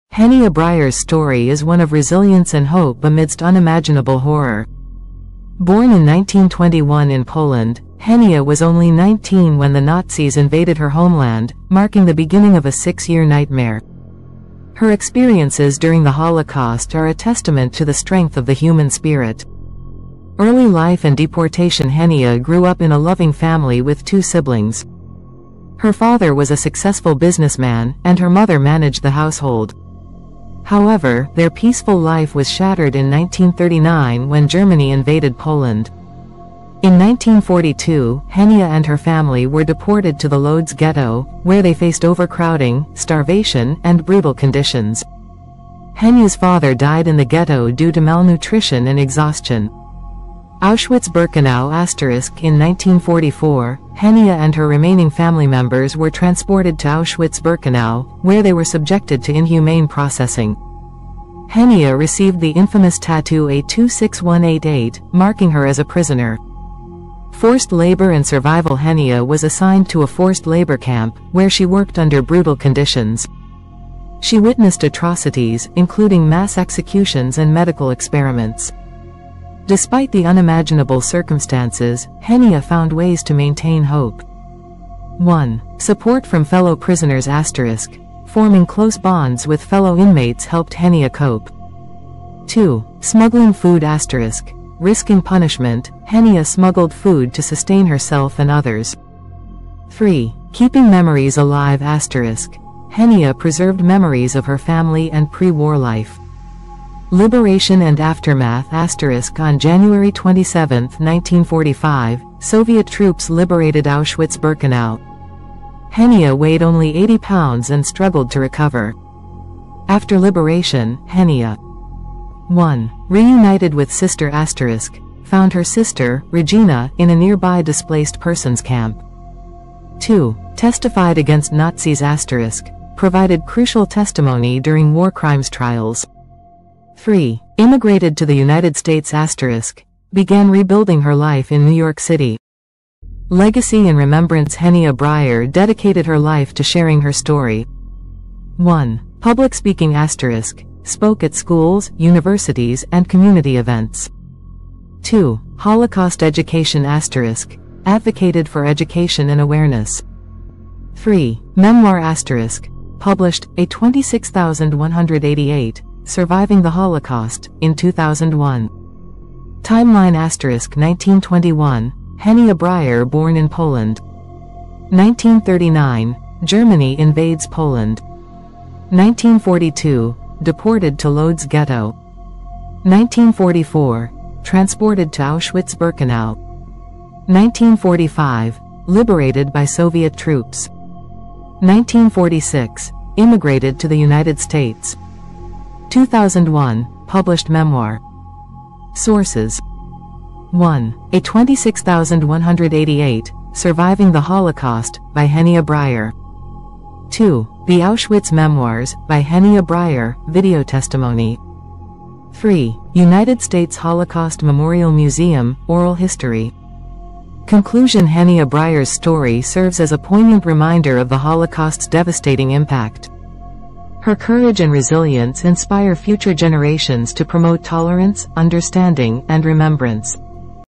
YOU WON'T BELIEVE She Survived the UNTHINKABLE: Henia Bryer's Holocaust Story
in this powerful video, we delve into the incredible story of Henia Bryer, a Holocaust survivor who faced unimaginable horrors and emerged with a remarkable spirit of resilience. Henia’s journey takes us from her early life in Poland to the harrowing experiences of hiding and loss during World War II.

Through her firsthand account, we explore the profound impact of the Holocaust on her life and the lessons she imparts about survival, hope, and the importance of remembrance. Join us as we honor her legacy and ensure that her story continues to resonate with future generations.

Together, we can keep the memories alive and foster understanding in our communities.